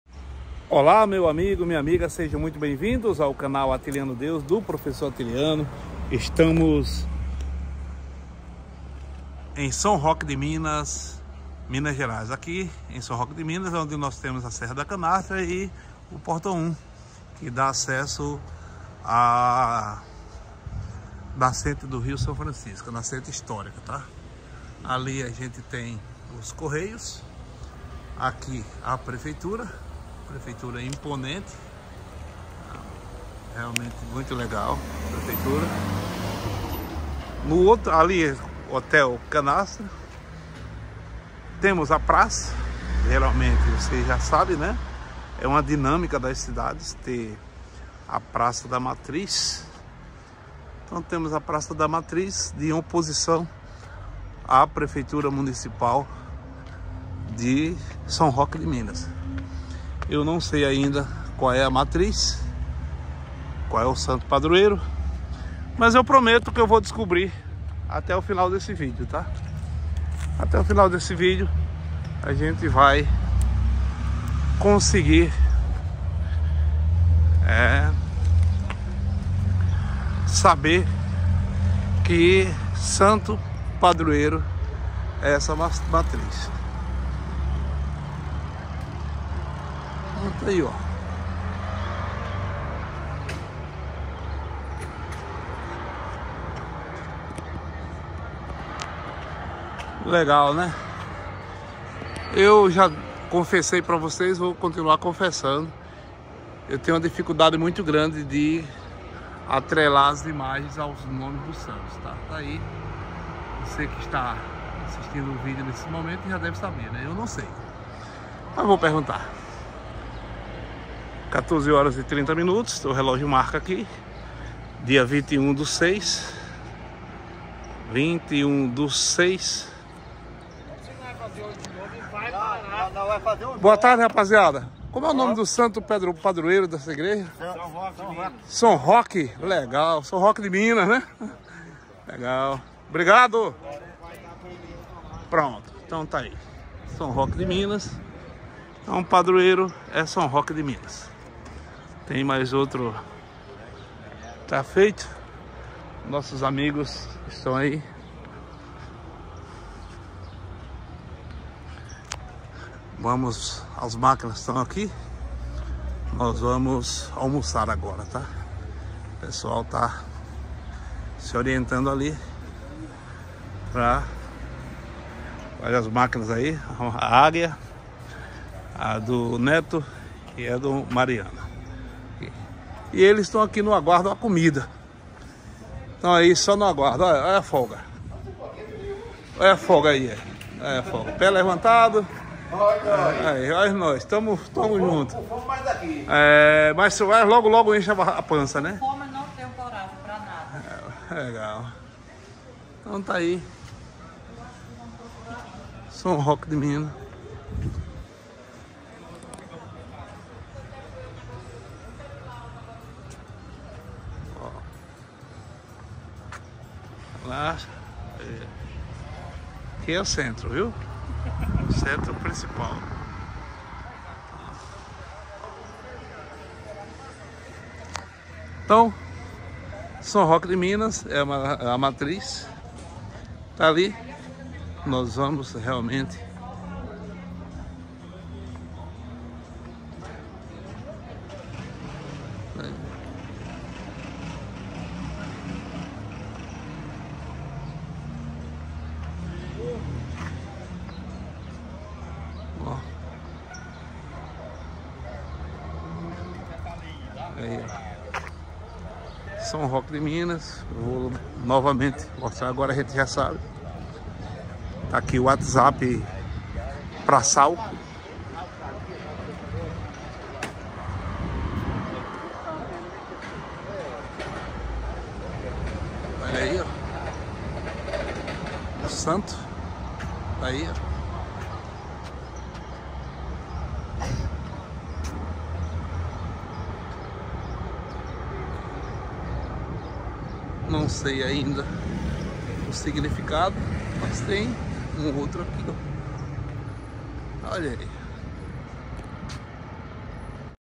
SÃO ROQUE DE MINAS/MG
Visitamos o bético rio São Francisco. Prof° Atiliano & Deise.